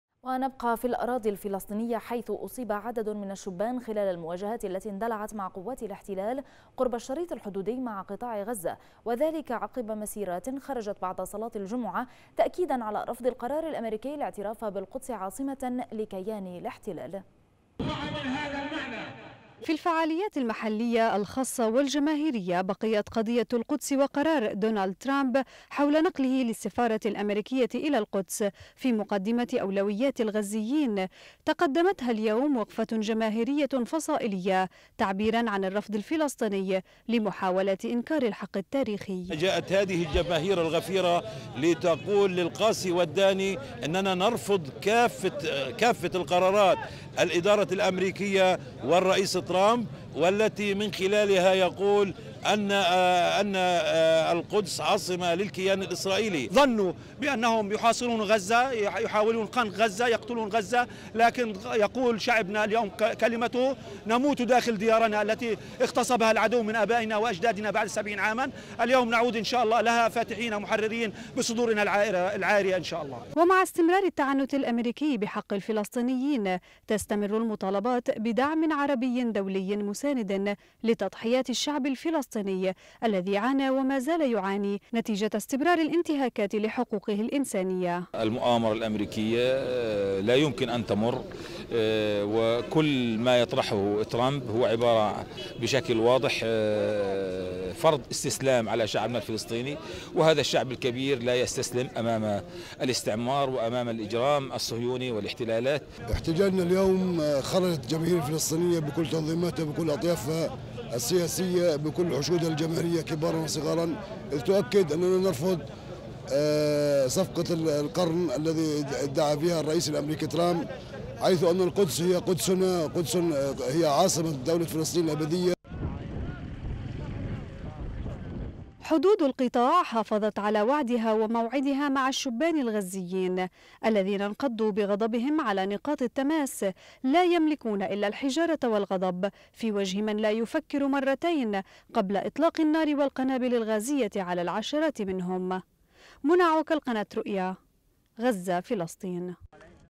عدد من الإصابات بين الفلسطينيين خلال مسيرات غضب في غزة - (9-3-2018)
أصيب عدد من الشبان خلال المواجهات التي اندلعت مع قوات الاحتلال قرب الشريط الحدودي مع قطاع غزة  وذلك عقب مسيرات خرجت بعد صلاة الجمعة تأكيدا على رفض القرار الامريكي الاعتراف بالقدس عاصمة لكيان الاحتلال.

تقرير: منى عوكل.